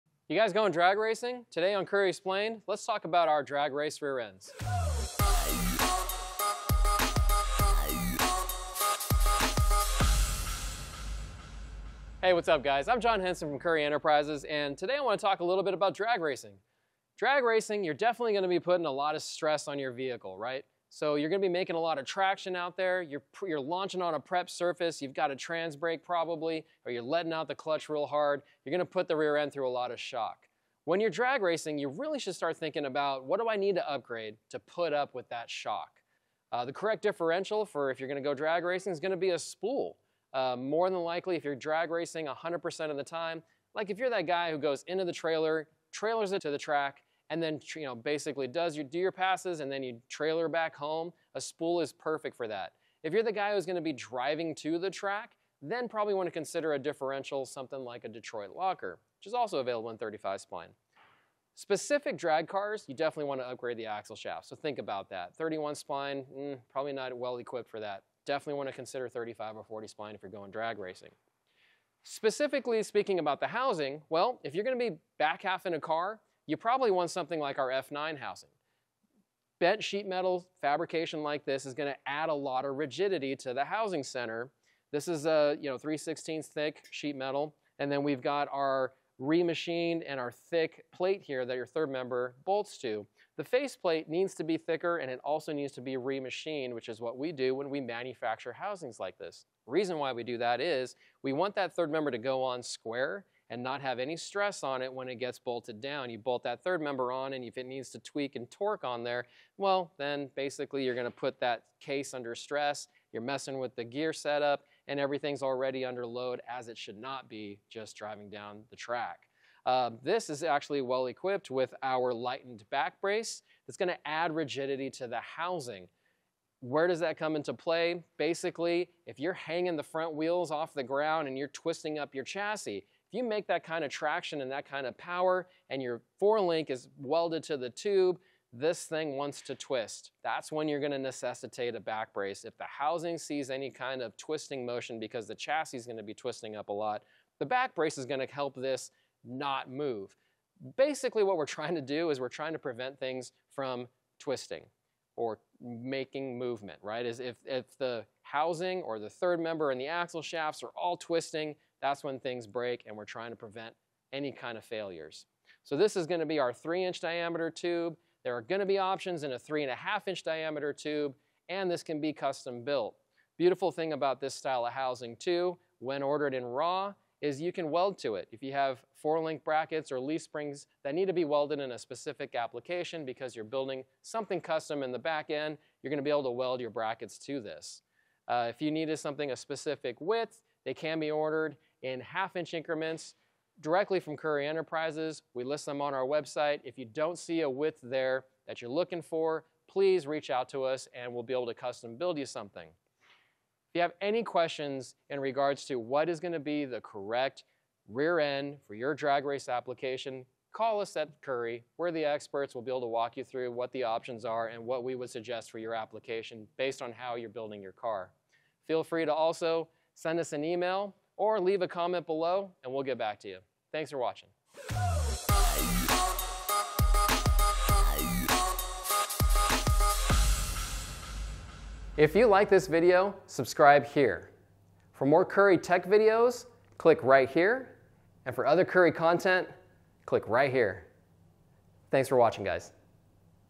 BUILDING A DRAG RACE CAR? YOU PROBABLY NEED THIS! | CURRIE 9-INCH DRAG RACE REAREND | EXPLAINED
Today on Explained, we are showing you the key features and options when building out a Drag Race Specific Currie 9-Inch rearend.